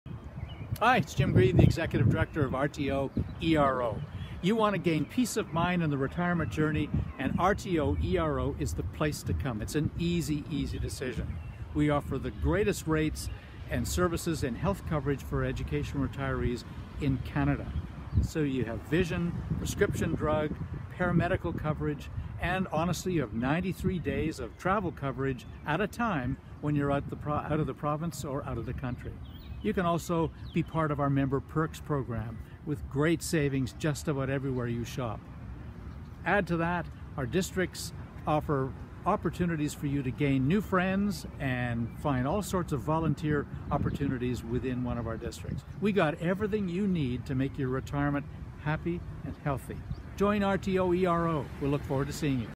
Why You Should Join RTO/ERO
Jim shares RTO/ERO's many membership benefits